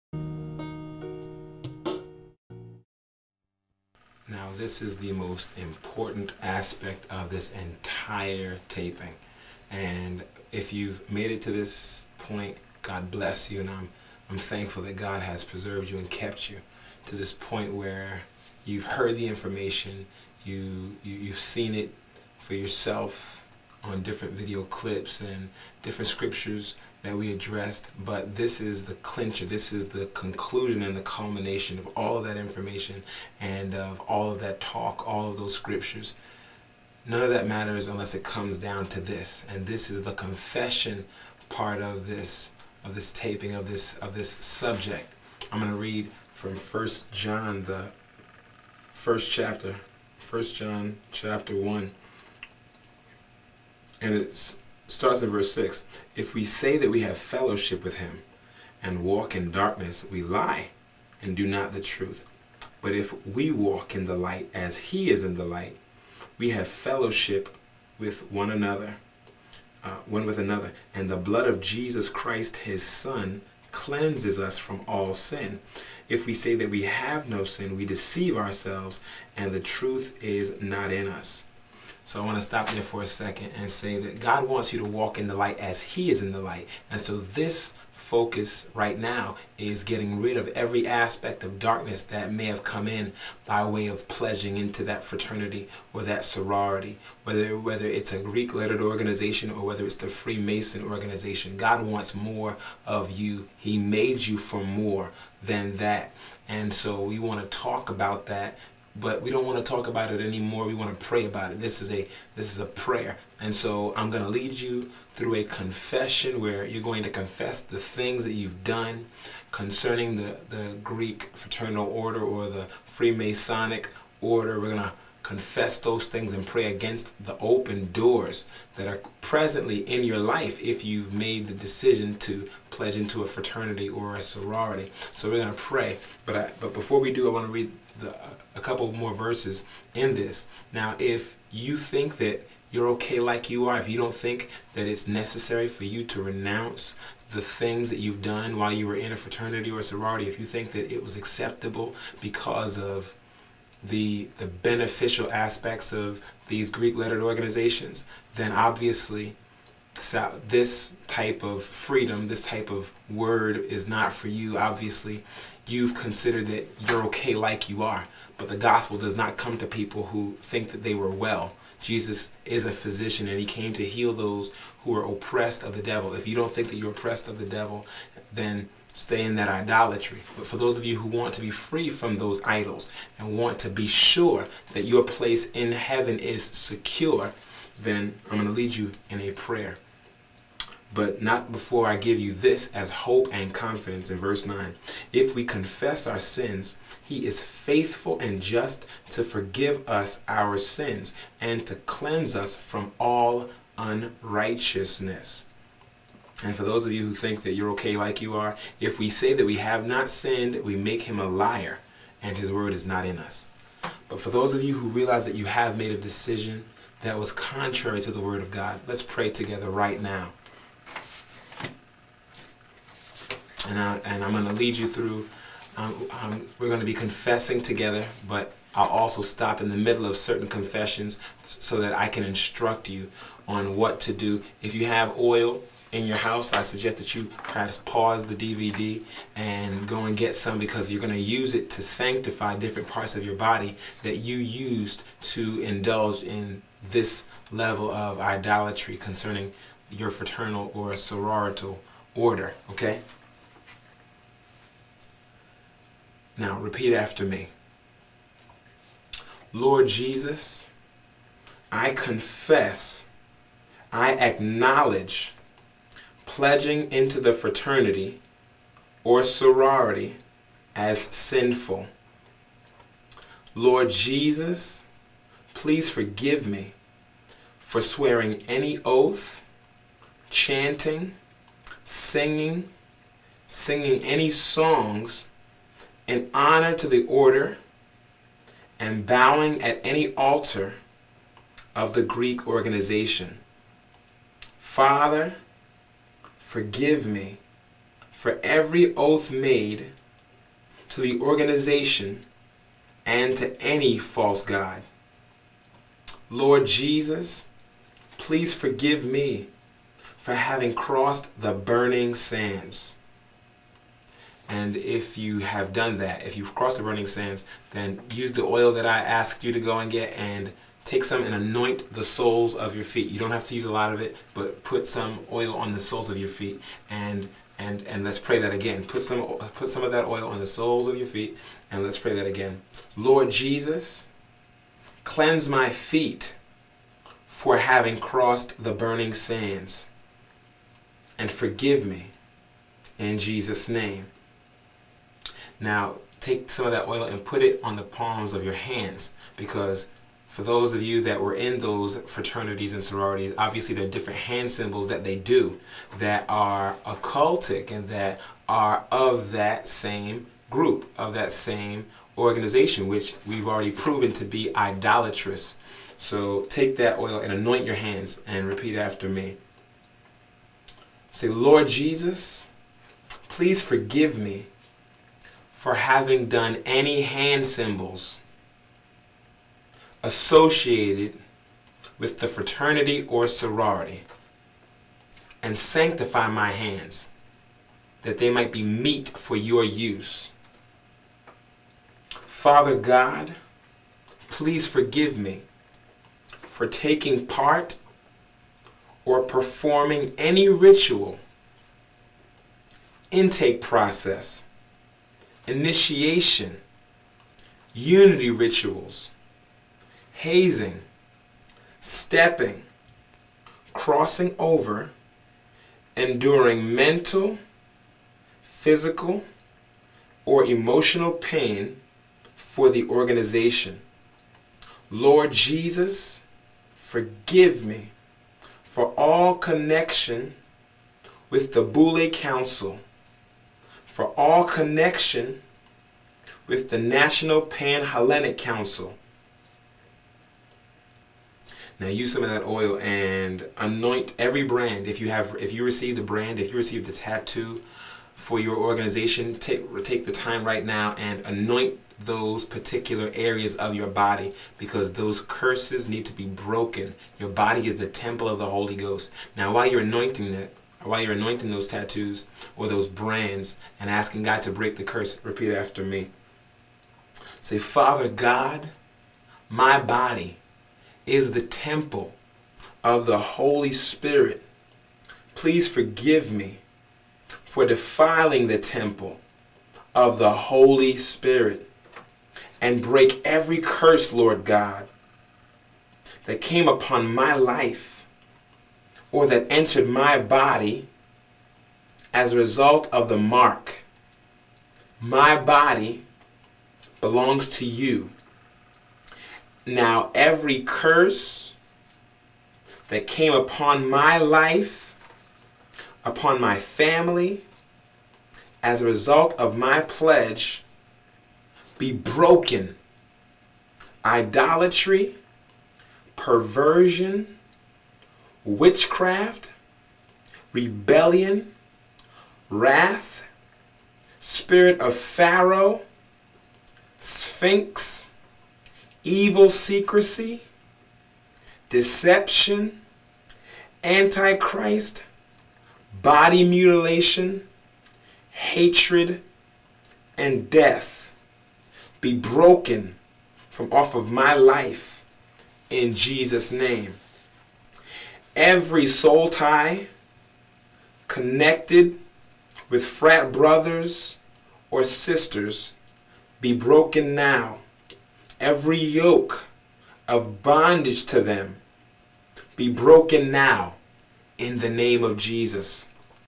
Real Talk 9: Prayer Breaking Curses on Black Greek Frat Members
Now that you've heard the truth, lets pray and make those who want freedom, free!